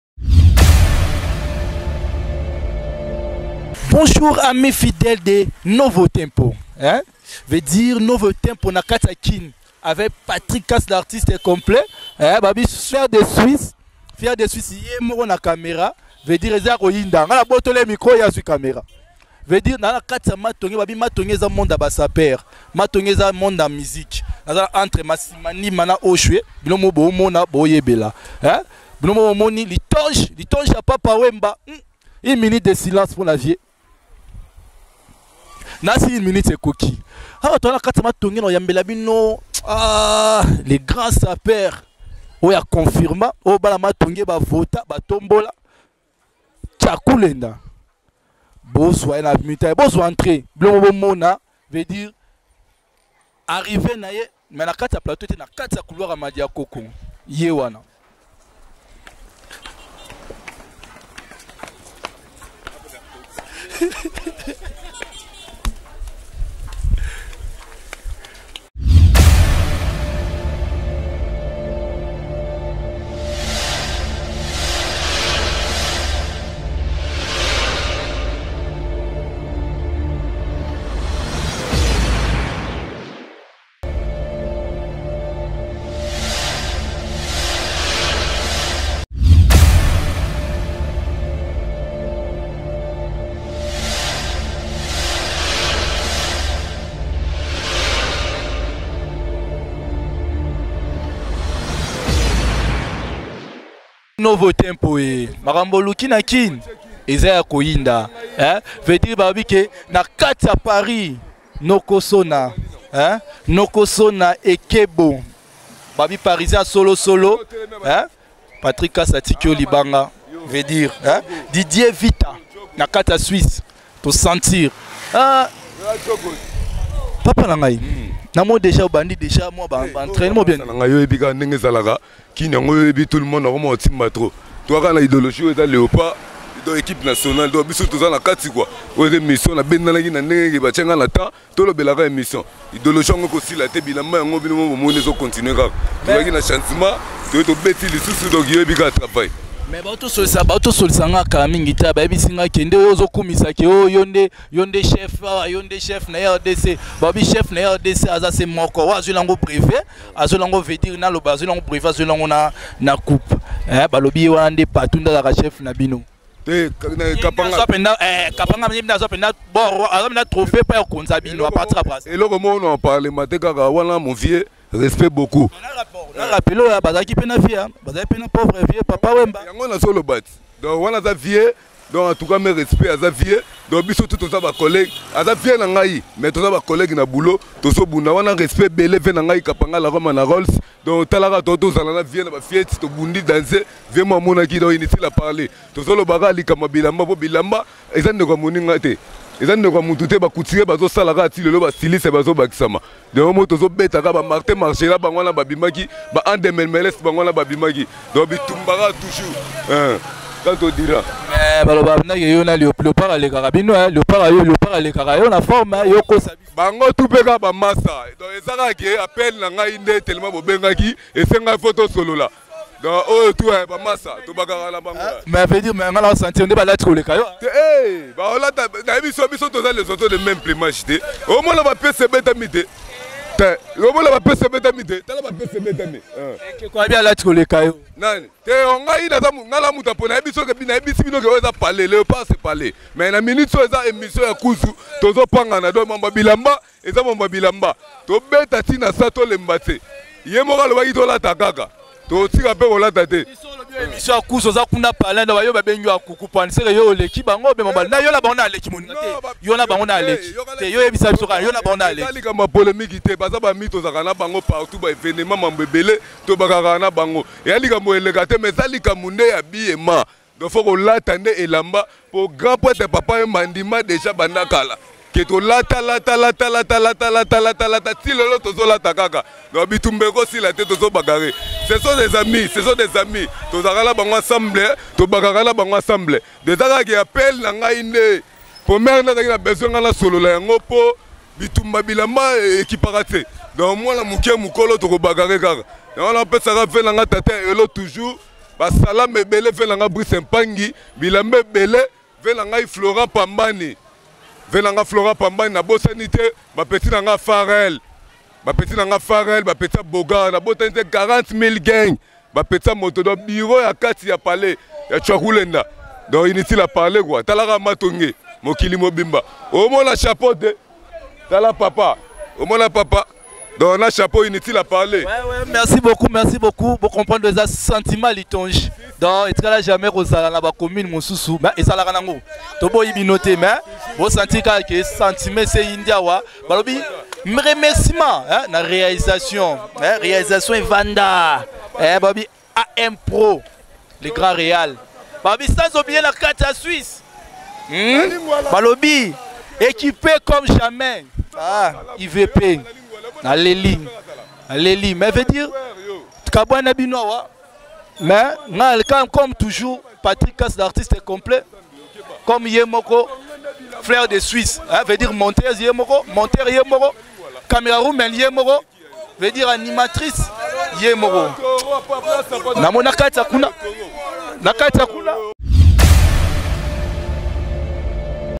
MUANA MOLOKAI TSHAKU LENDA Akimisi LEOPARD DE LA SAPE
AFFRONTEMENT NA COULOIR MADIAKOKO SEUL CONTRE TOUS
TSHAKU LENDA AZO DEFENDRE MOLOKAI.